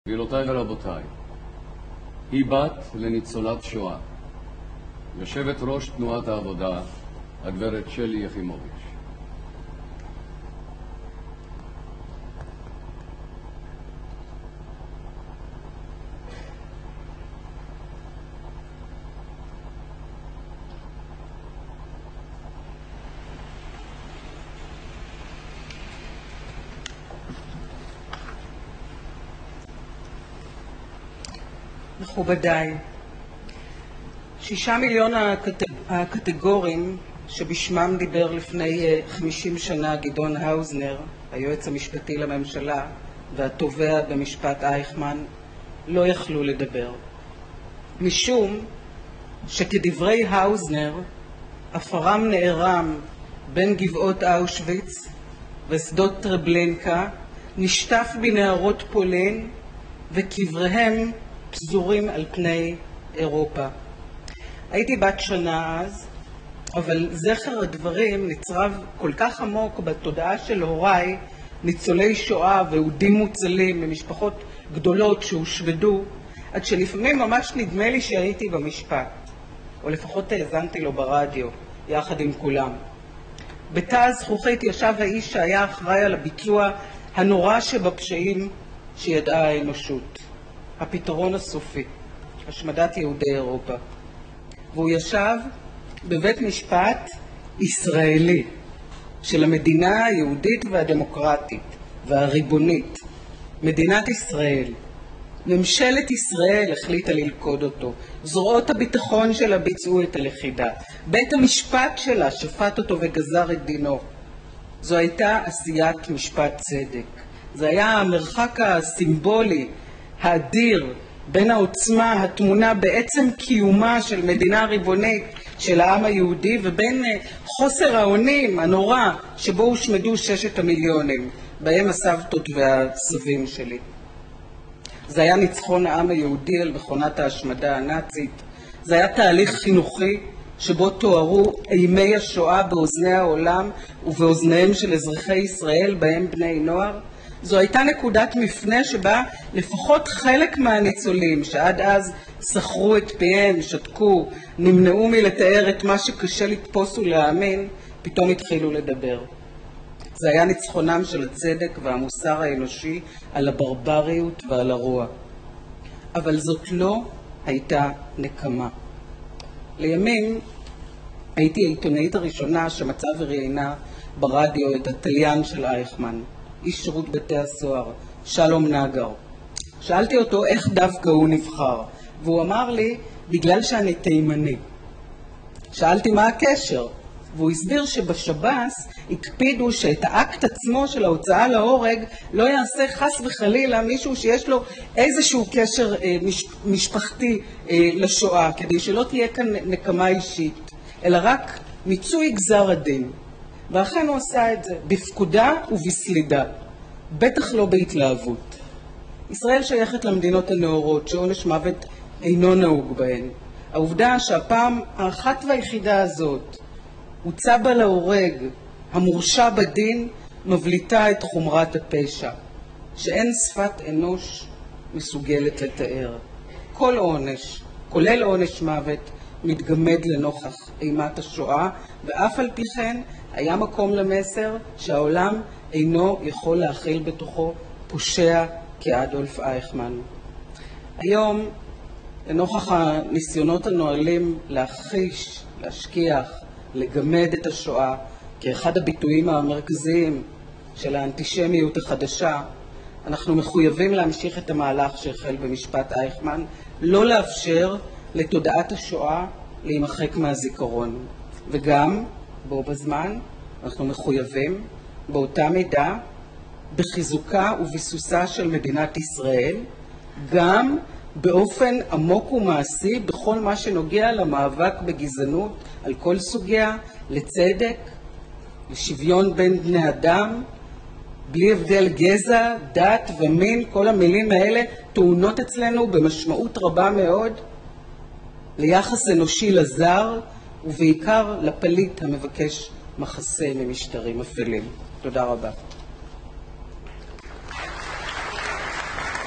שלי יחימוביץ'- נאום באירוע שנת ה-50 למשפט אייכמן
יו"ר מפלגת העבודה ח"כ שלי יחימוביץ' בנאום באירוע שנת ה-50 למשפט אייכמן: "היום לנוכח הניסיונות הנואלים להכחיש, להשכיח ולגמד את השואה, כאחד הביטויים המרכזיים של האנטישמיות החדשה, אנו מחויבים להמשיך את המהלך שהחל במשפט אייכמן -- לא לאפשר לתודעת השואה להימחק מן הזיכרון.
וגם בו בזמן, בצד חיזוקה וביסוסה של מדינת ישראל, להיות מחויבים באופן עמוק וגם מעשי למאבק בגזענות על כל סוגיה, לצדק, לשוויון בין בני אדם בלא הבדל גזע דת ומין, וליחס אנושי לזר, ובעיקר לפליט המבקש מחסה ממשטרים אפלים."